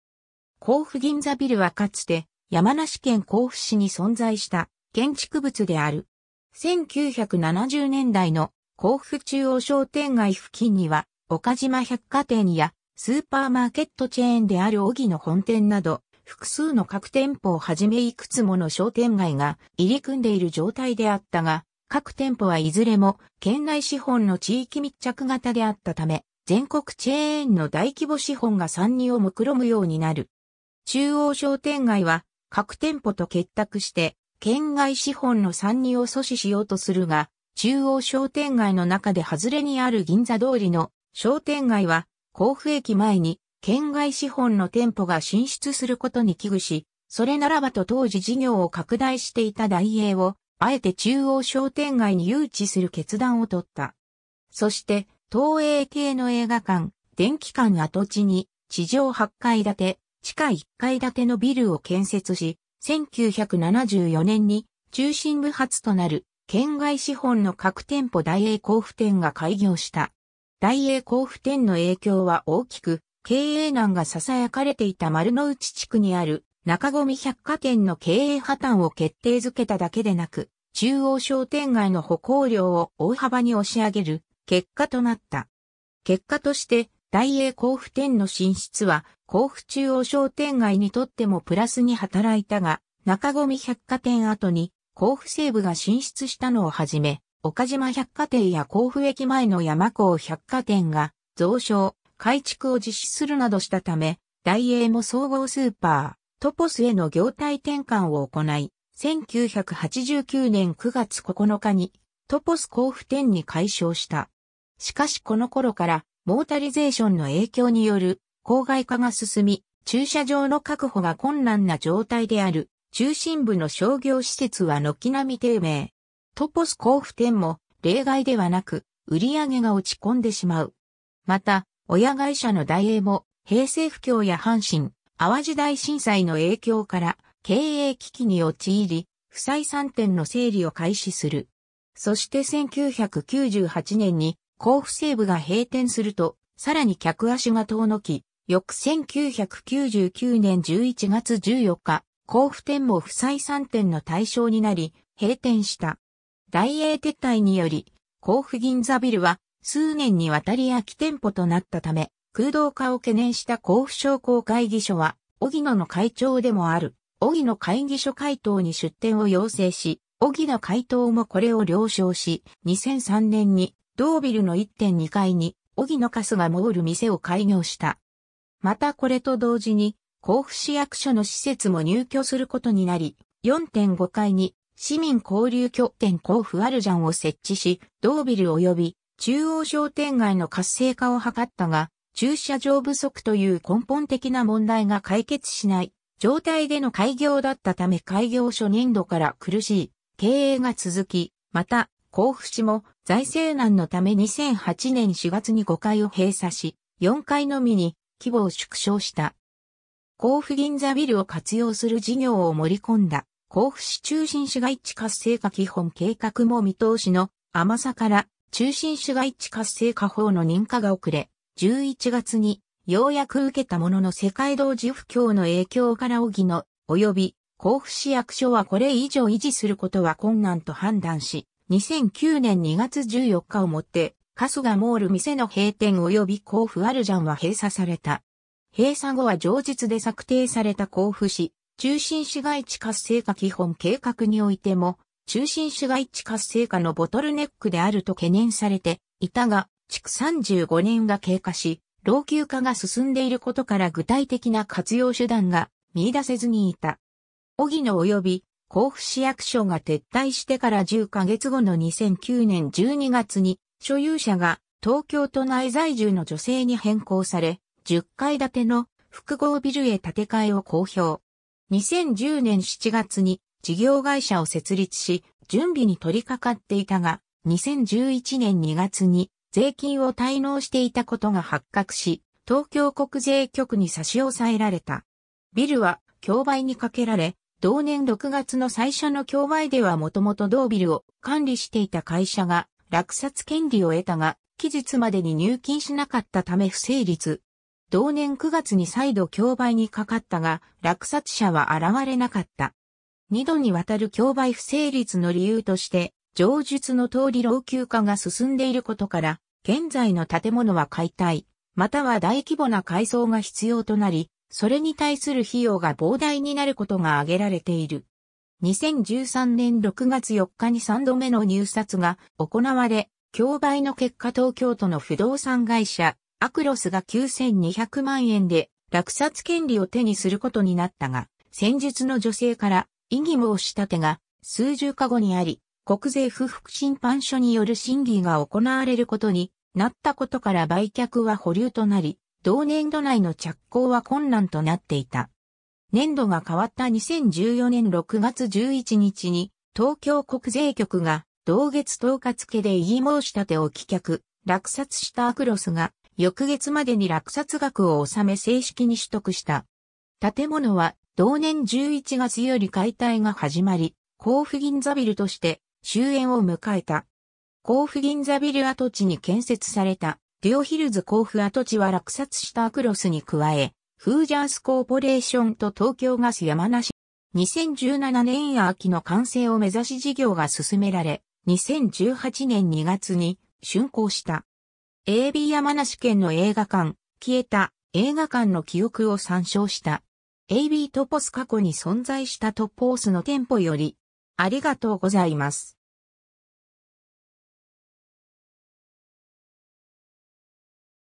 甲府銀座ビル
甲府銀座ビル（こうふぎんざビル）はかつて山梨県甲府市に存在した建築物（ビルディング）である。
 1970年代の甲府中央商店街付近には岡島百貨店やスーパーマーケットチェーンであるオギノ本店など複数の核店舗をはじめいくつもの商店街が入り組んでいる状態であったが、核店舗はいずれも県内資本の地域密着型であったため、全国チェーンの大規模資本が参入を目論むようになる。
中央商店街は核店舗と結託して県外資本の参入を阻止しようとするが、中央商店街の中で外れにある銀座通りの商店街は甲府駅前に県外資本の店舗が進出することに危惧し、それならばと当時事業を拡大していたダイエーをあえて中央商店街に誘致する決断を取った。
そして東映系の映画館「電気館」跡地に地上8階建、地下1階建のビルを建設し、1974年（昭和49年）に中心部初となる県外資本の核店舗ダイエー甲府店（愛称：甲府ショッパーズプラザ）が開業した。
 ダイエー甲府店の影響は大きく、経営難が囁かれていた丸の内地区にある中込百貨店の経営破綻を決定付けただけでなく、中央商店街の歩行量を大幅に押し上げる結果となった。
結果としてダイエー甲府店の進出は甲府中央商店街にとってもプラスに働いたが、中込百貨店跡に甲府西武が進出したのをはじめ、岡島百貨店や甲府駅前の山交百貨店が増床・改築を実施するなどしたため、ダイエーも総合スーパー「トポス」への業態転換を行ない、1989年（平成元年）9月9日にトポス甲府店に改称した。
 しかしこの頃からモータリゼーションの影響による郊外化が進み、駐車場の確保が困難な状態である中心部の商業施設は軒並み低迷。
トポス甲府店も例外ではなく、売上げが落ち込んでしまう。
また親会社のダイエーも平成不況や阪神・淡路大震災の影響から経営危機に陥り、不採算店の整理を開始する。
そして1998年（平成10年）に甲府西武が閉店するとさらに客足が遠のき、翌1999年（平成11年）11月14日、甲府店も不採算店の対象になり、閉店した。
 ダイエー撤退により甲府銀座ビルは数年にわたり空き店舗となったため、空洞化を懸念した甲府商工会議所はオギノの会長でもある荻野会議所会頭に出店を要請し、荻野会頭もこれを了承し、2003年（平成15年）に同ビルの1・2階にオギノかすがもーる店を開業した。
またこれと同時に甲府市役所の施設も入居することになり、4・5階に市民交流拠点こうふアルジャンを設置し、同ビルおよび中央商店街の活性化を図ったが、駐車場不足という根本的な問題が解決しない状態での開業だったため開業初年度から苦しい経営が続き、また甲府市も財政難のため2008年（平成20年）4月に5階を閉鎖し、4階のみに規模を縮小した。
甲府銀座ビルを活用する事業を盛り込んだ甲府市中心市街地活性化基本計画も見通しの甘さから中心市街地活性化法の認可が遅れ、11月にようやく受けたものの世界同時不況の影響からオギノおよび甲府市役所はこれ以上維持することは困難と判断し、2009年（平成21年）2月14日を以てかすがもーる店の閉店およびこうふアルジャンは閉鎖された。
 閉鎖後は上述で策定された甲府市中心市街地活性化基本計画においても「中心市街地活性化のボトルネックである」と懸念されていたが、築35年が経過し老朽化が進んでいることから具体的な活用手段が見いだせずにいた。
 オギノおよび甲府市役所が撤退してから10ヵ月後の2009年12月に所有者が東京都内在住の女性に変更され、10階建ての複合ビルへ建て替えを公表。
2010年7月に事業会社を設立し準備に取り掛かっていたが、2011年2月に税金を滞納していたことが発覚し、東京国税局に差し押さえられた。
ビルは競売にかけられ、同年6月の最初の競売では元々同ビルを管理していた会社が落札権利を得たが期日までに入金しなかったため不成立。
同年9月に再度競売にかかったが落札者は現れなかった。
2度にわたる競売不成立の理由として上述の通り老朽化が進んでいることから現在の建物は解体または大規模な改装が必要となり、それに対する費用が膨大になることが挙げられている。
 2013年6月4日に3度目の入札が行われ、競売の結果東京都の不動産会社「アクロス」（現在の名前。当時は「シーアンドスタイル」）が9200万円で落札権利を手にすることになったが、先述の女性から異議申立てが数十日後にあり、国税不服...